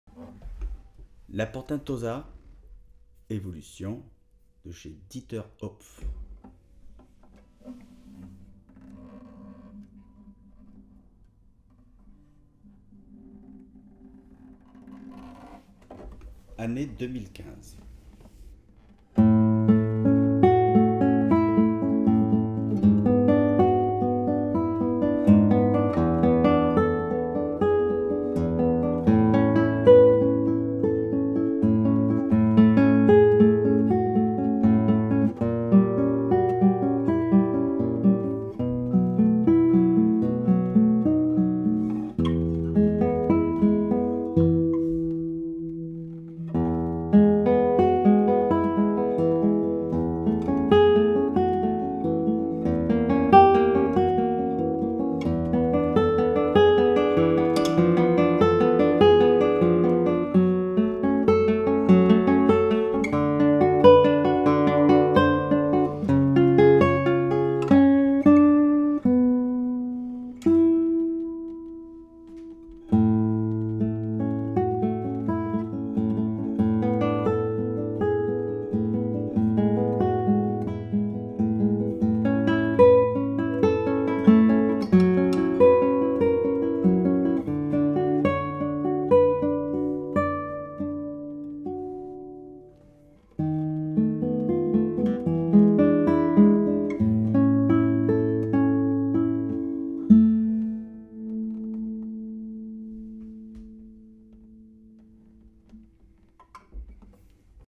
2015 DIETER HOPF PORTENTOSA EVOLUCION LATTICE CLASSICAL GUITAR – demo CARCASSI
2015 DIETER HOPF PORTENTOSA EVOLUCION LATTICE CLASSICAL GUITAR –   demo CARCASSI

The famous Portentosa has been declined under the lattice format by Dieter Hopf. This new Portentosa called Evolucion represents the achievement in terms of lattice  classical guitarmaking : power, musicality, balance and confort. If all the Portentosa models looks the same, this Evolution is a true revolution in terms of lutherie. A fantastic instrument!
This lattice classical guitar is a dream : the best selling Portentosa has been revisted with a lattice top. The sound remains typical of Dieter Hopf classical guitars but with more power and more definition. Very fat treebles with dark basses. A very 'romantic' or 'spanish' sound but with huge overtones and long sustain. The neck is perfect. One of the best guitar in the world.
An outstanding guitar that will give great pleasure. A real benchmark.
The most exquisite double-top and lattice concert guitars.